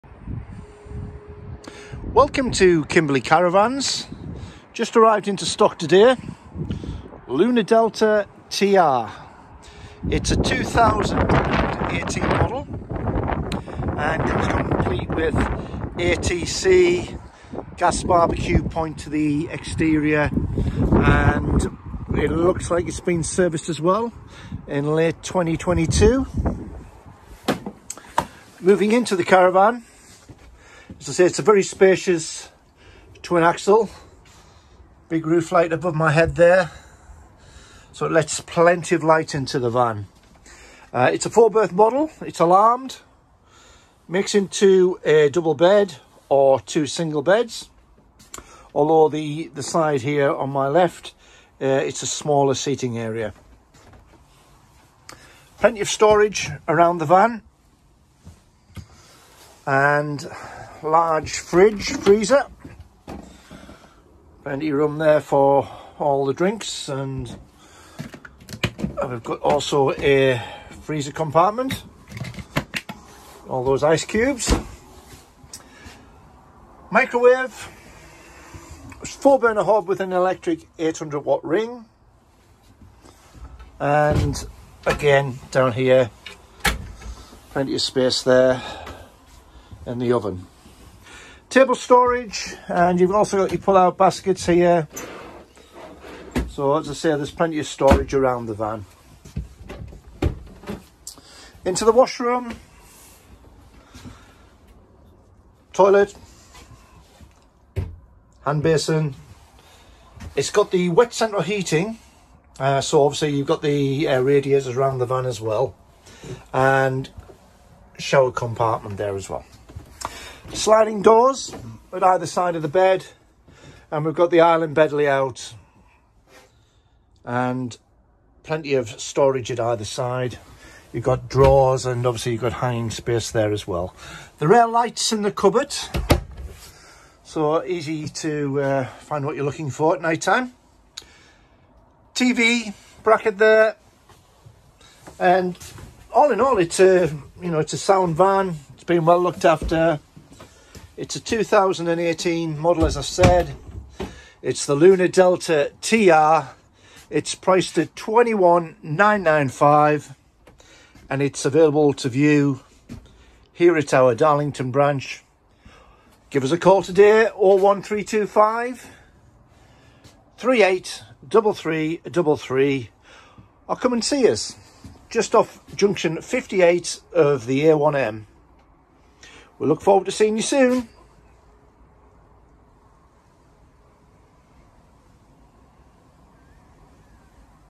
2018 Lunar Delta TR
The Lunar Delta TR - spacious & bright, all in one and we are proud to have it available at our Darlington branch.
Here David is showing you around this fantastic vehicle which boasts ALDE heating system, ALKO ATC & alarm system which sleeps 4.
All of this for only £21995!!
Book in a viewing today or call up one of our welcoming and helpful salesmen at Darlington on: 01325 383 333.